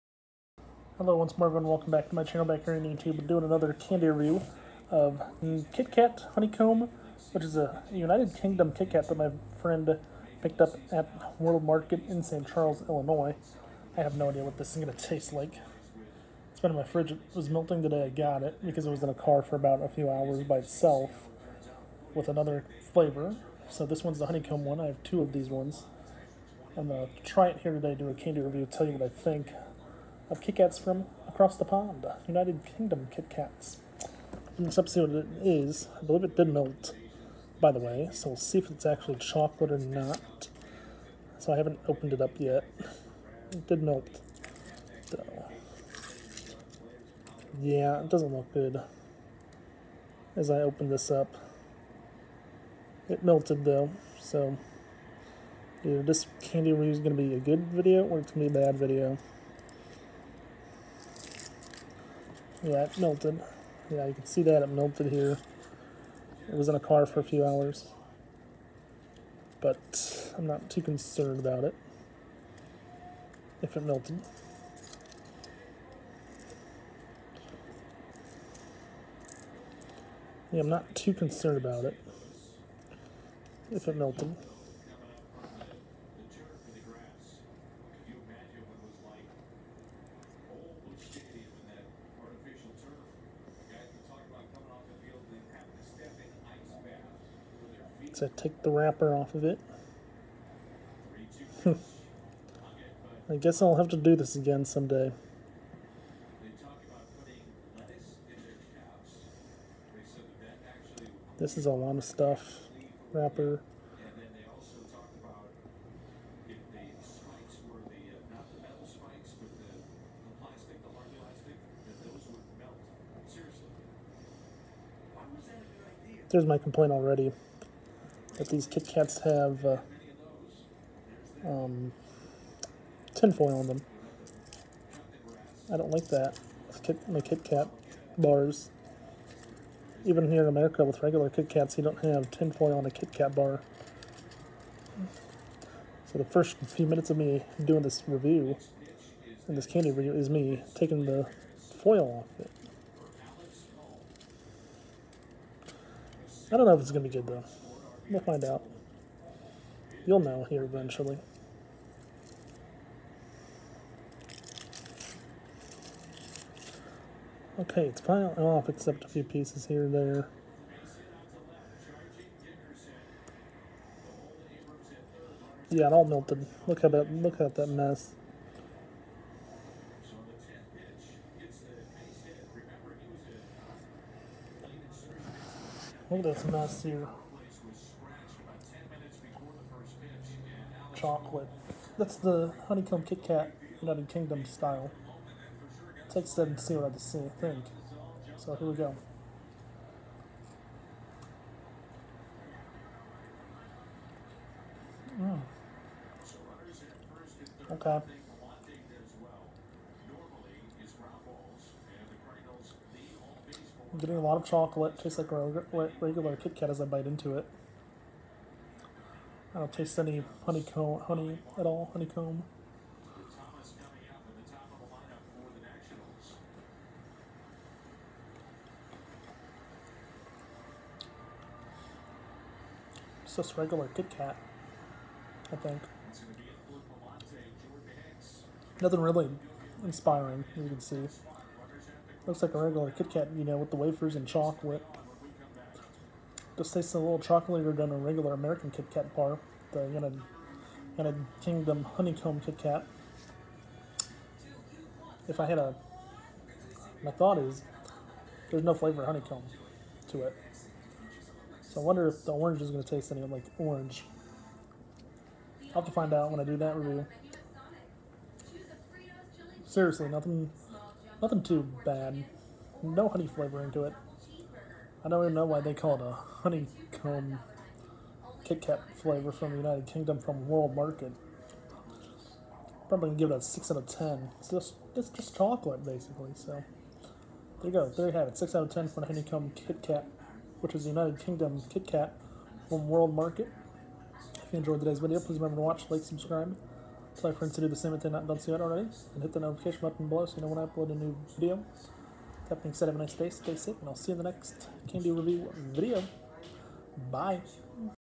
UK Honeycomb KitKat
Here is the newest candy review video of UK KitKat Honeycomb that my friend @goombajoey picked up at world market. Nothing to special no honey flavor a little sweeter of a chocolate. Overall I have to give it a 6 out of 10. I hope everyone enjoys the video.